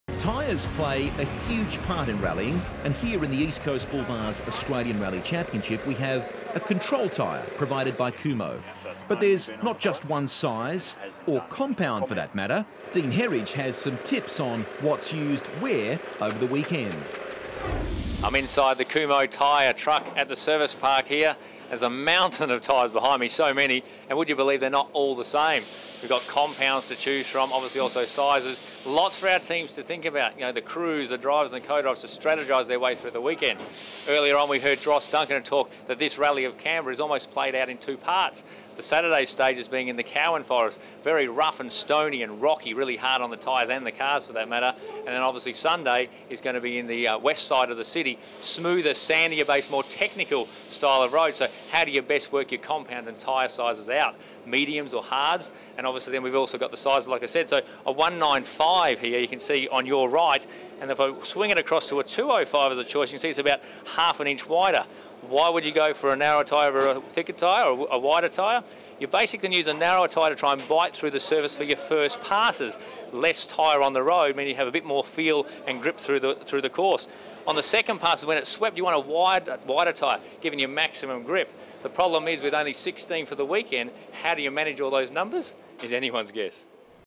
Kumho Tech Talk - Tyre Selection
Tyres are a big part of rallying and with Kumho providing the control tyre, they come to each rally with a very large truck filled with all the variations of tyre required for all of our competitors.